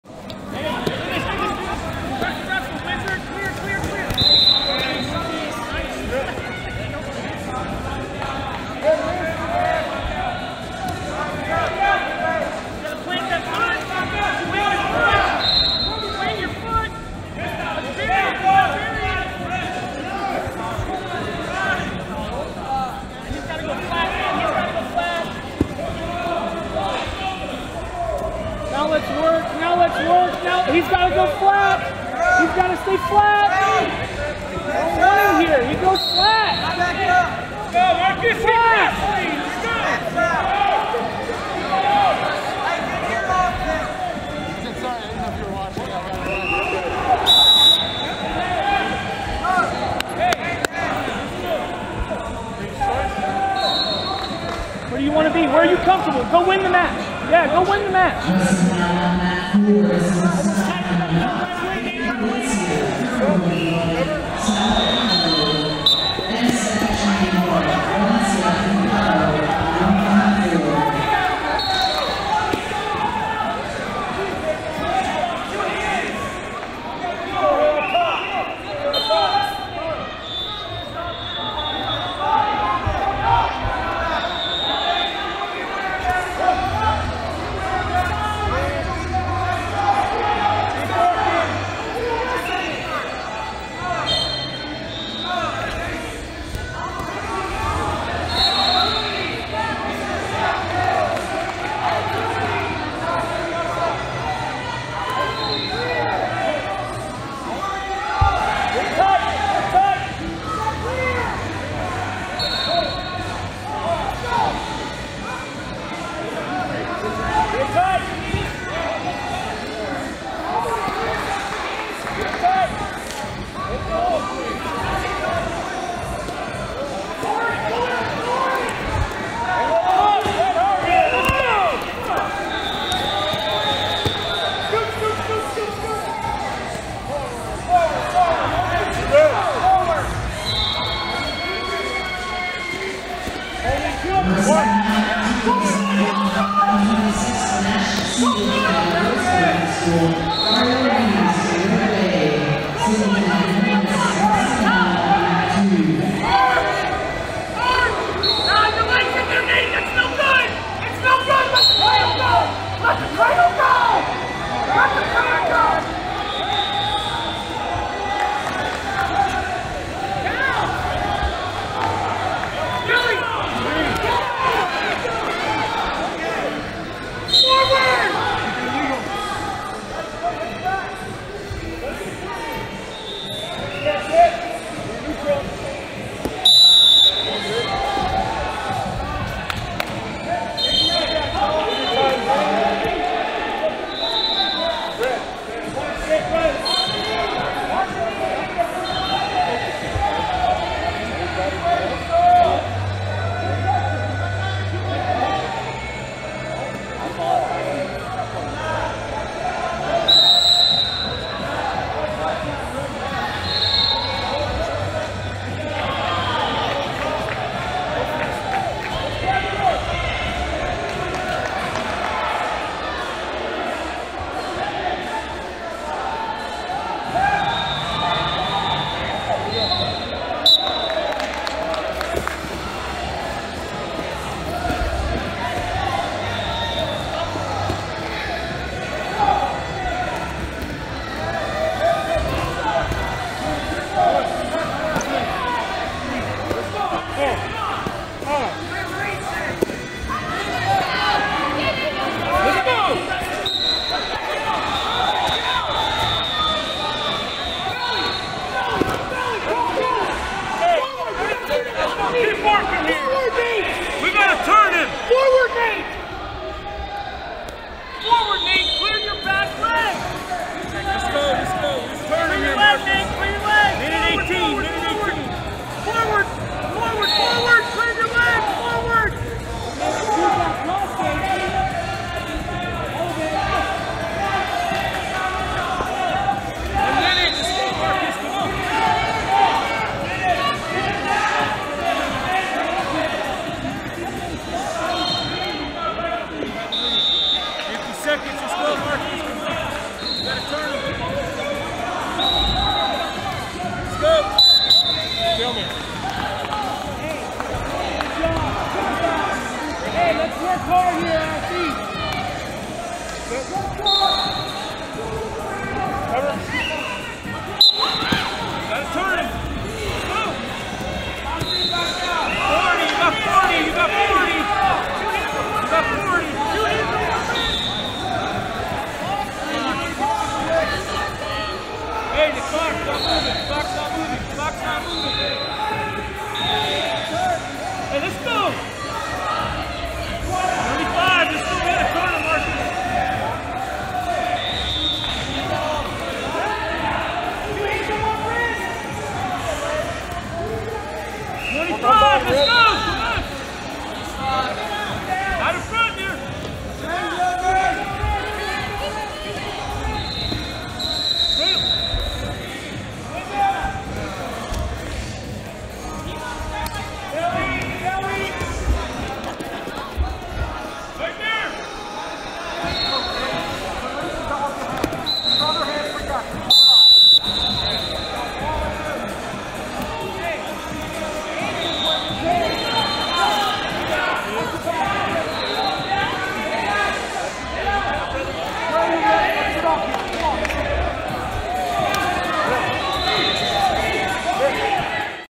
106 – Marcus Williams {G} of Rio Rancho NM vs. Nate Smith {R} of Southport IN
March 27, 2021 — USA Wrestling High School National Recruiting Showcase – Coralville, IA

Nate Smith {R} of Southport IN defeated Marcus Williams {G} of Rio Rancho NM by 11–4